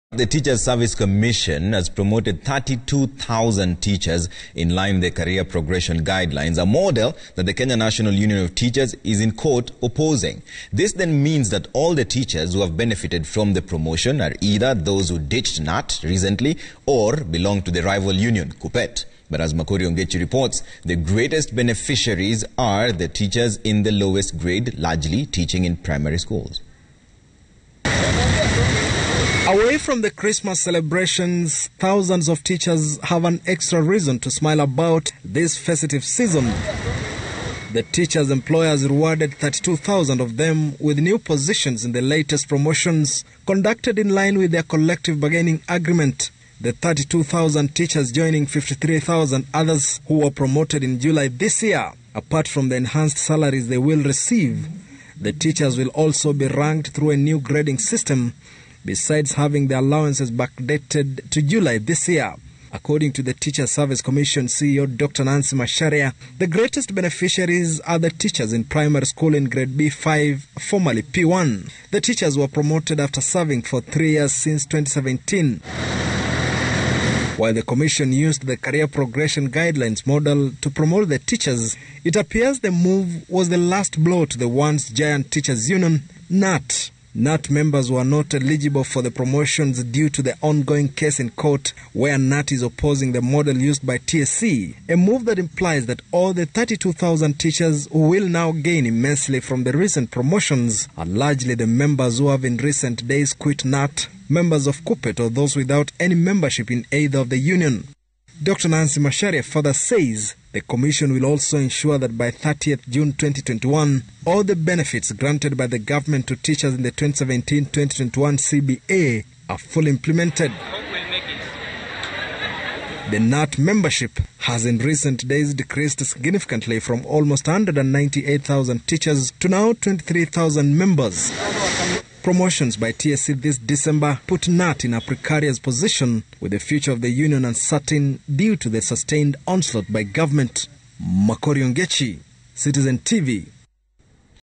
Teachers who decamped from Sossion led union get promotions
TSC has promoted 32,000 teachers most of whom were KNUT members
New promotions bring to 84,000 the total number of teachers promoted this year
Macharia: Promotions target teachers in primary schools on grade B5